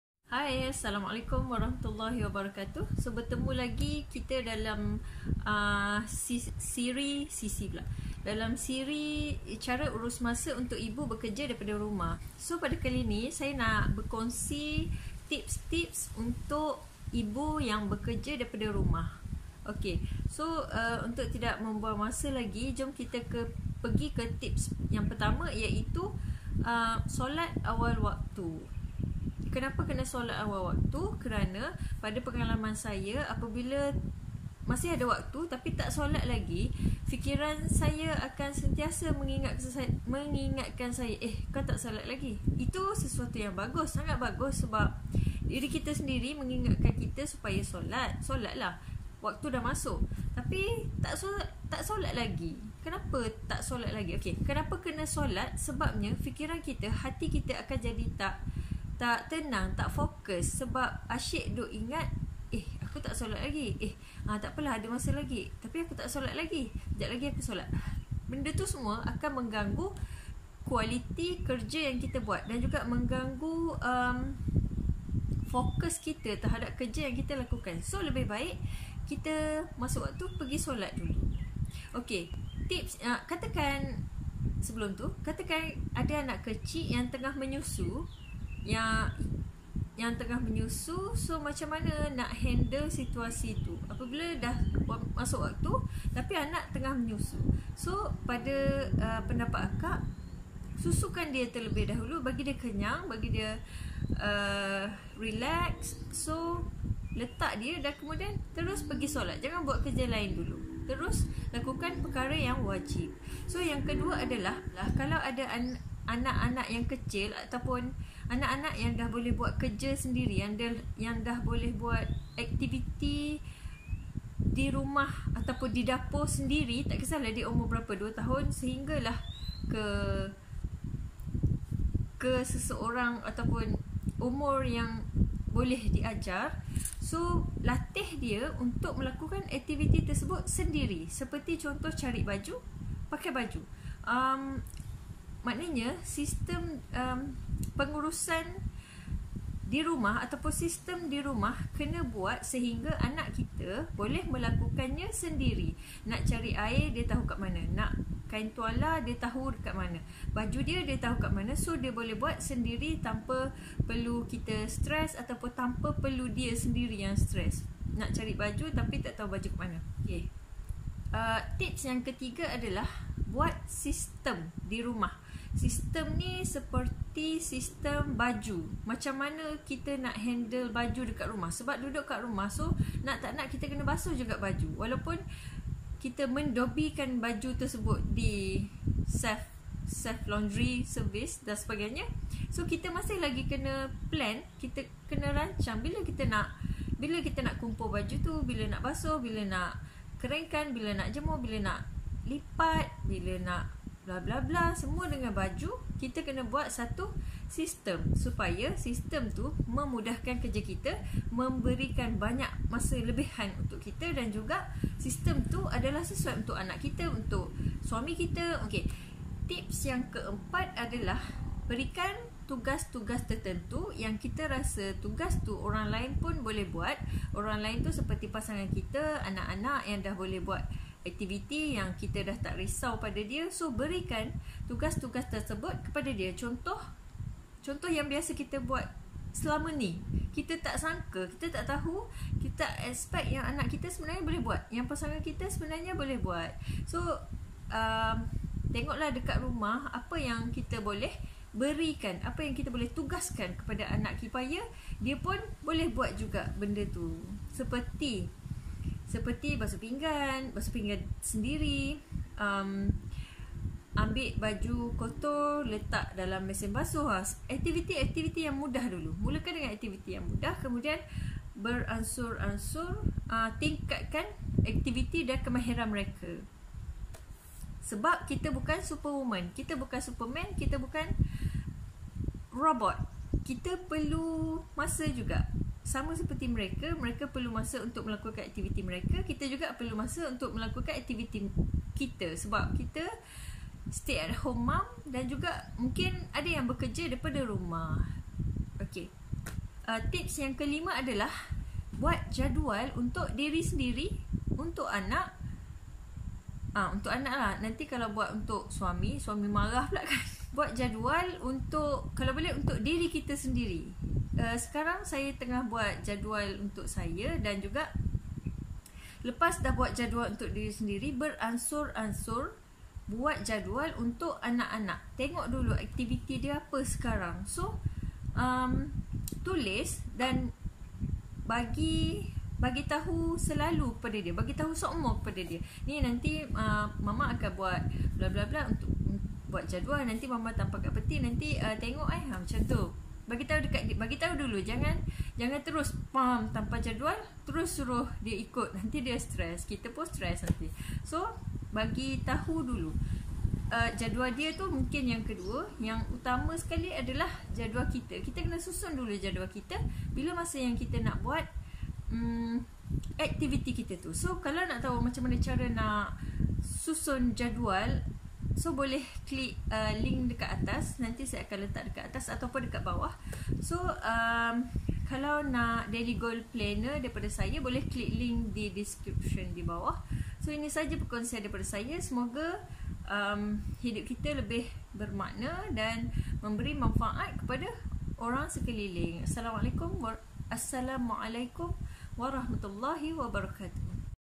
Tips Untuk Ibu Bekerja Dari Rumah | cara dapatkan masa lebih
Antara tips yang boleh saya kongsikan di sini. Ada 5.

1. Solat awal waktu
2. Ajar anak buat kerja/aktiviti rumah yang kita rasa dia boleh buat seperti amik dan pakai baju sendiri
3. Buat sistem seperti sistem baju/meal olan dan sebagainya
4. Berikan tugas atau bahagikan tugas kepada anak anak yang dah boleh buat kerja rumah (menyapu/ basuh pinggan)
5. Buat jadual atau time blok sendiri dan anak anak

Nak daily goal planner Percuma? Klik sini

Semoga membantu para ibu, suri rumah, permaisuri rumah tangga dan ibu bekerja di pejabat juga.

Like komen dan share sekiranya bermanfaat.